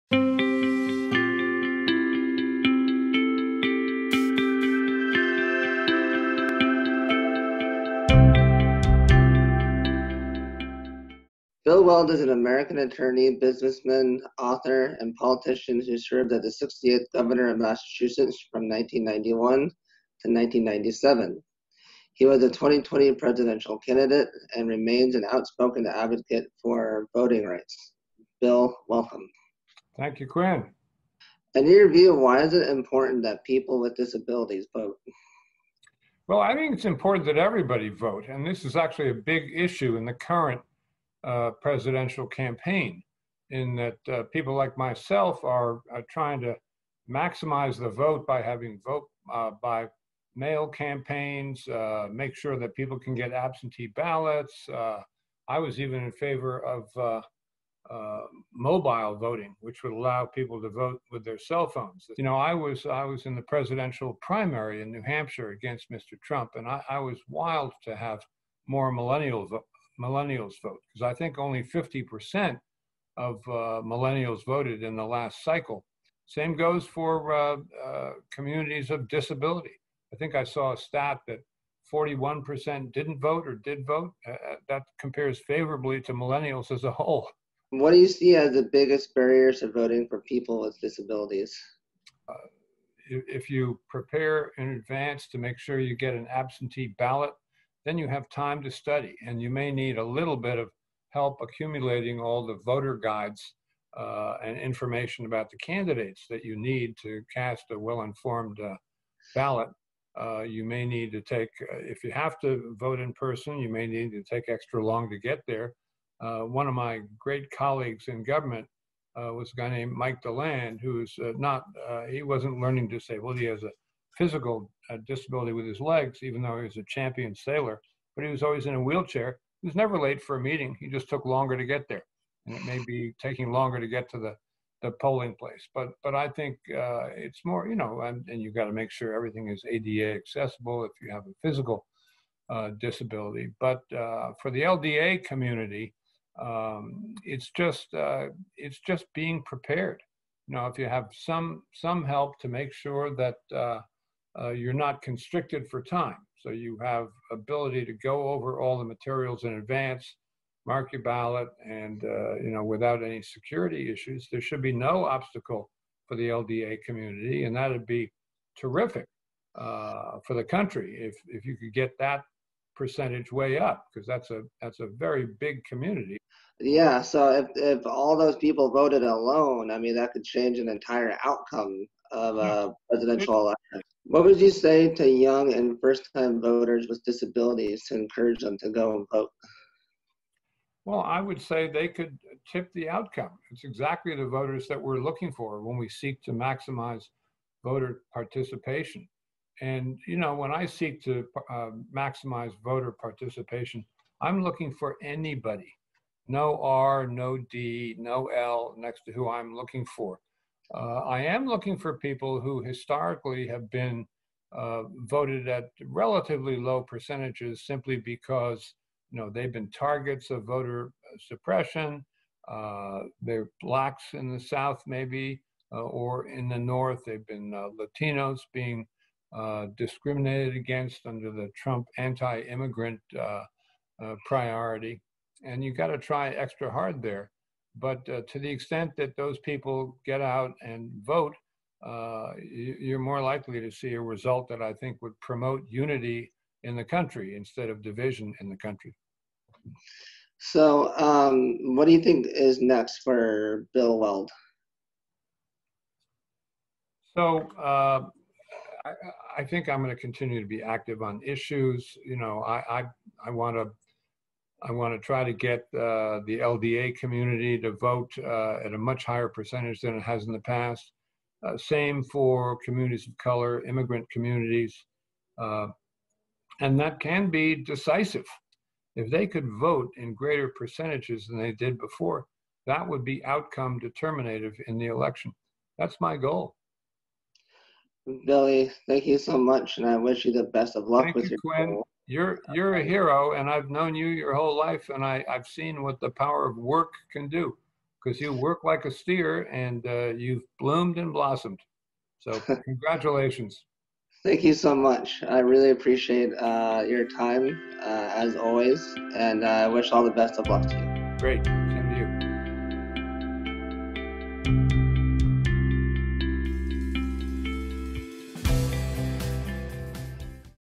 Governor Bill Weld & Quinn Bradlee
Quinn sits down with 2020 Presidential Candidate and former Massachusetts Governor Bill Weld to talk about why it is so important for people with disabilities to get out the vote.